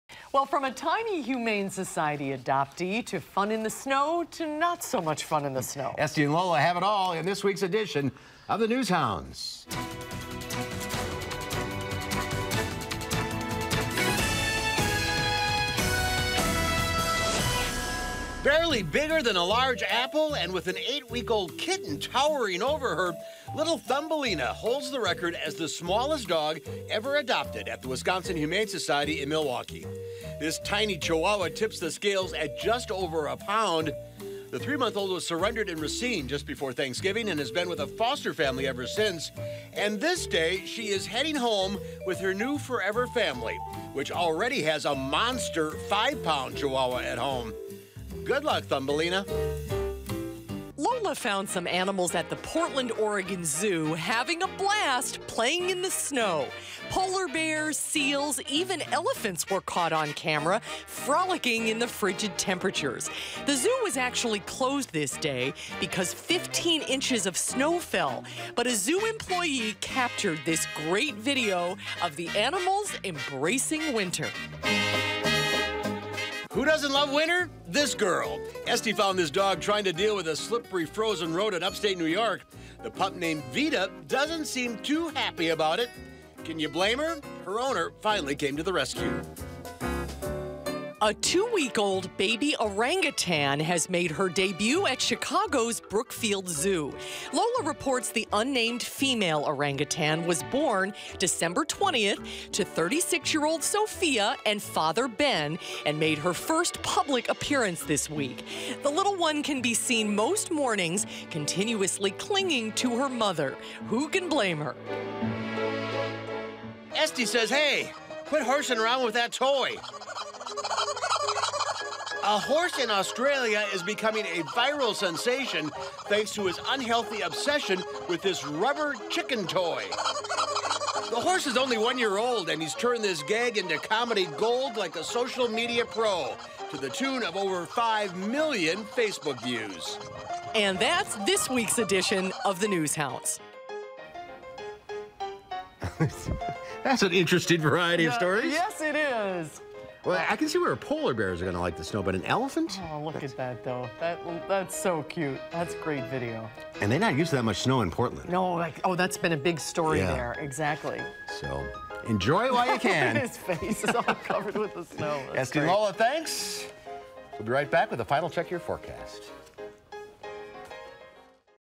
News hounds: Tiny chihuahua
Estie and Lola bring us the latest in animal news.